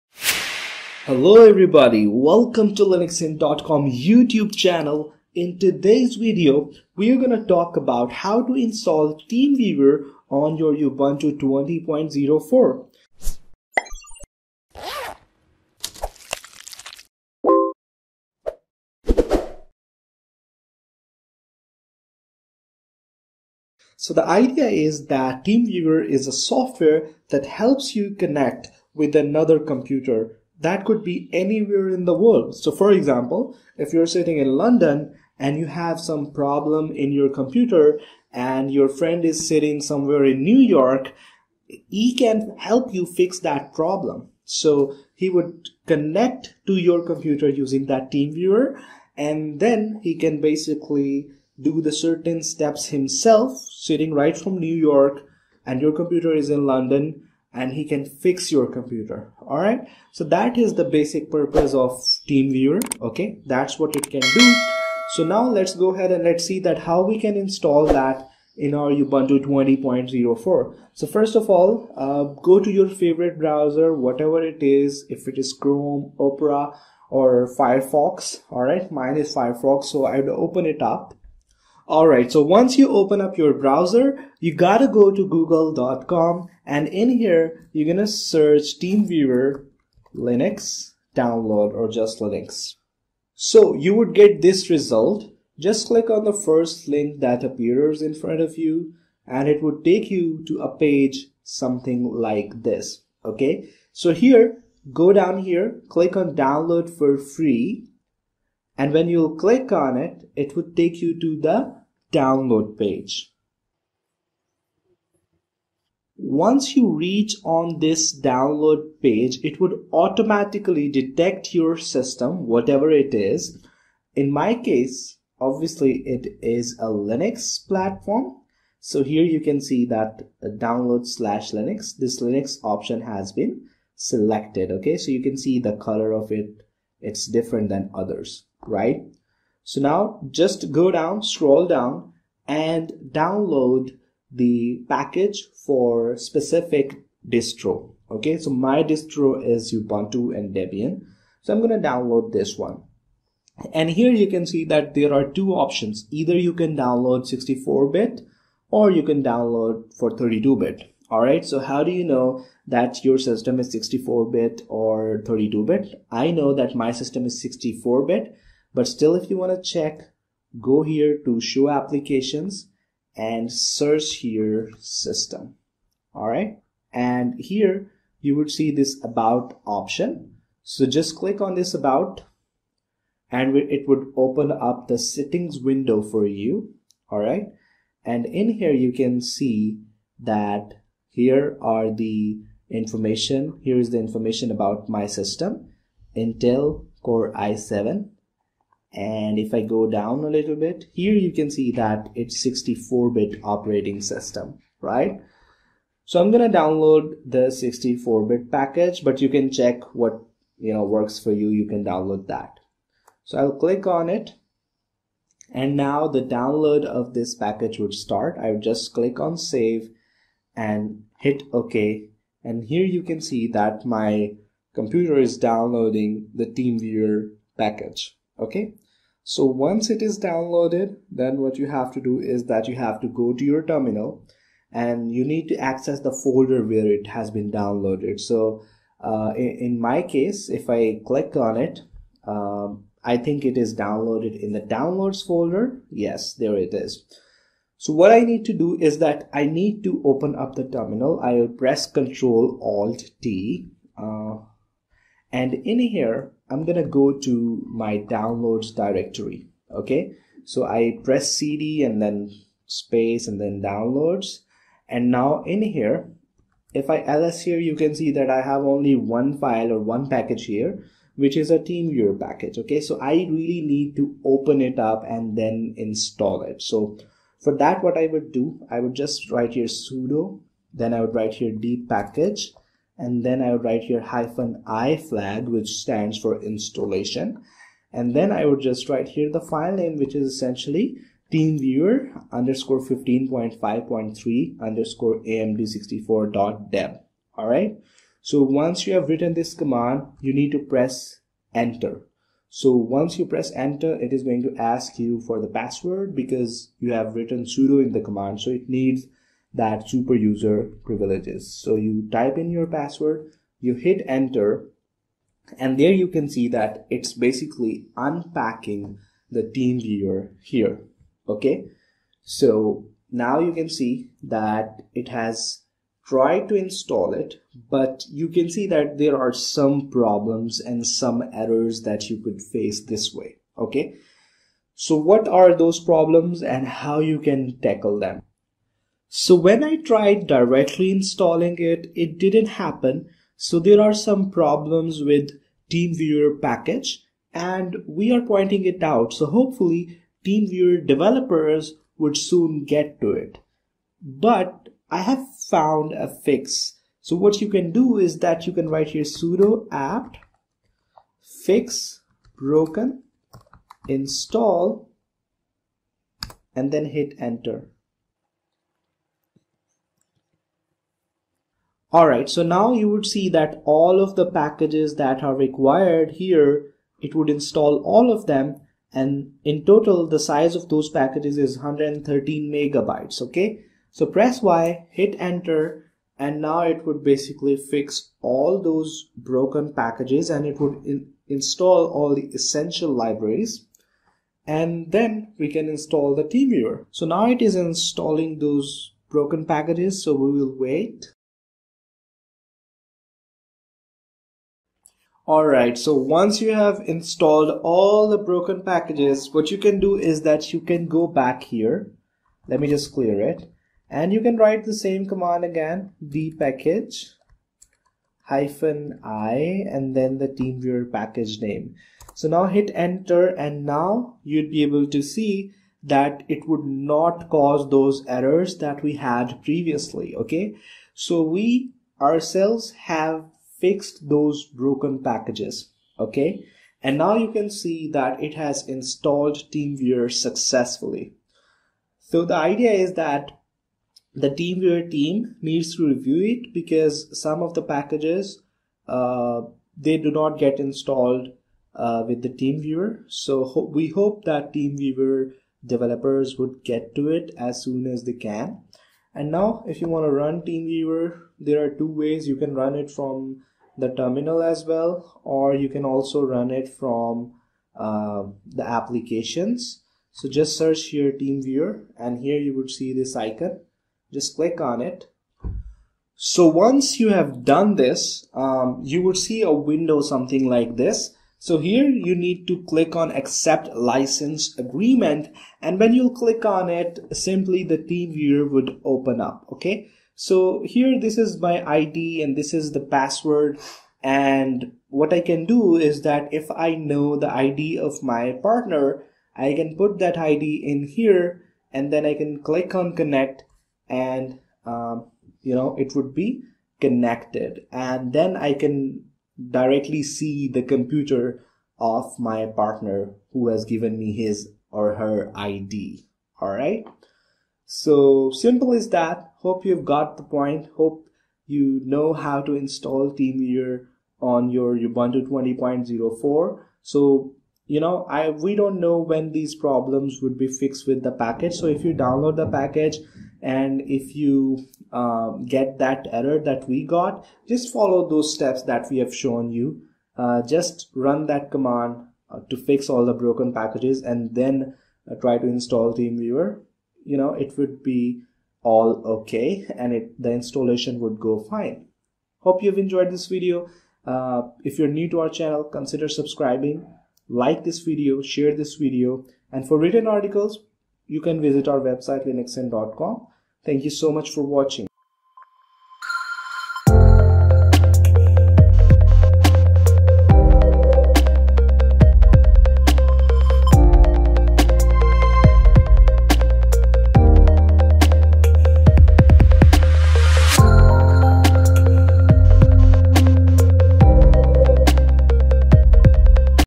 How to install TeamViewer on Ubuntu 20.04
In this video, We will show you how to install TeamViewer. TeamViewer is a tool specialized for connecting to other computers remotely that are out of your physical reach. For example, if you are sitting in United States and your friend is in China and your friend has some problem with his or her computer then you can basically connect to his or her computer directly by sitting in US and make changes in his or her computer thousands of miles away.